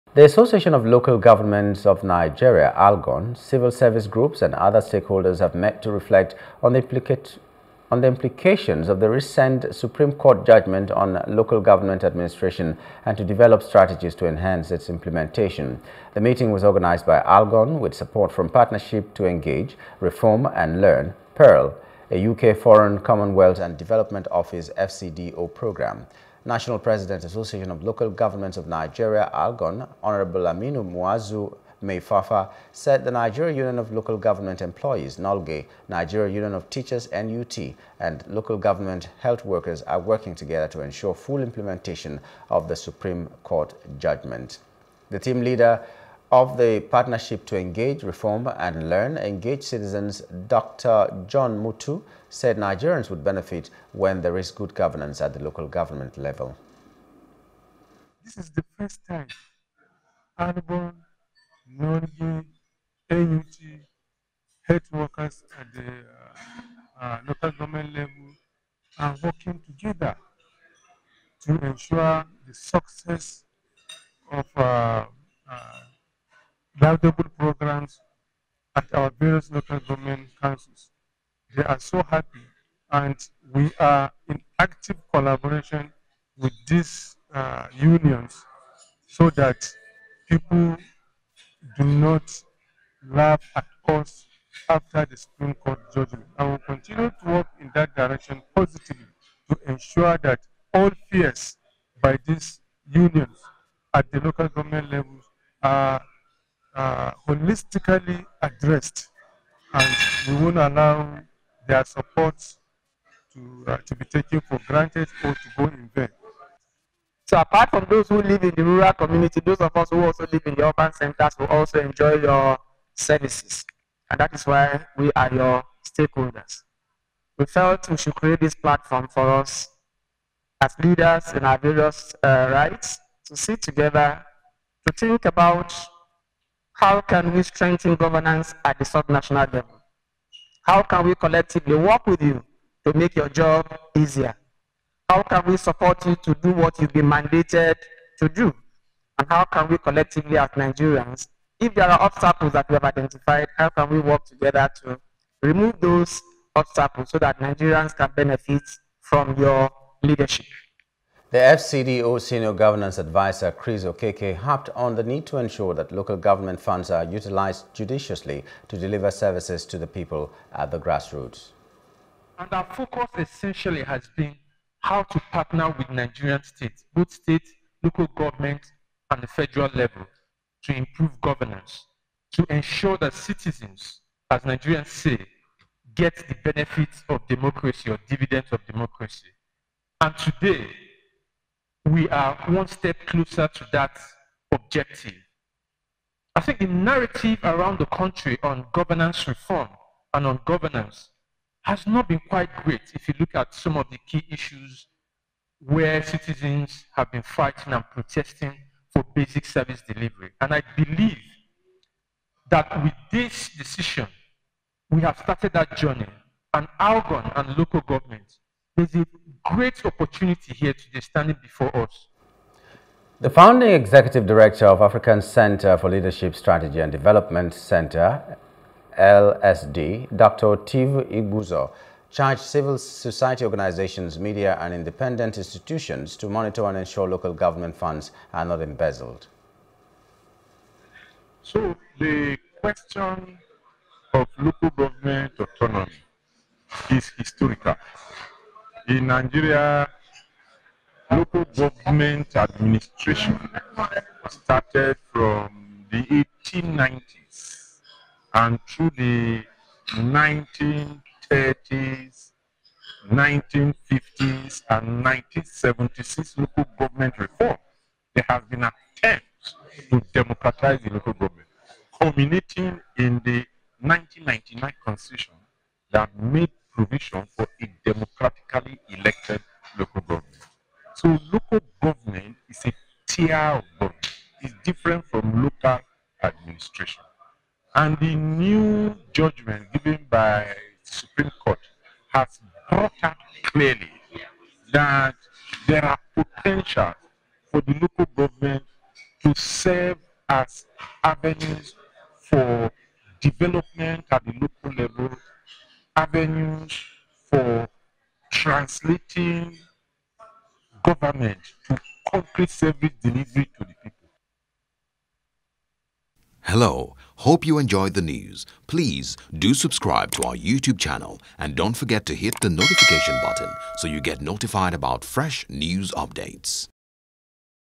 LG Administration: Algon, Stakeholders Reflect On Implications Of Supreme Court Judgement
The Association of Local Governments of Nigeria (ALGON), Civil Society Groups and other stakeholders have met to reflect on the implications of the recent Supreme Court judgement on Local Government administration and to develop strategies to enhance its implementation.
The meeting was organised by ALGON with support from Partnership to Engage, Reform and Learn (PERL), a UK, Foreign, Commonwealth, and Development Office (FCDO) programme.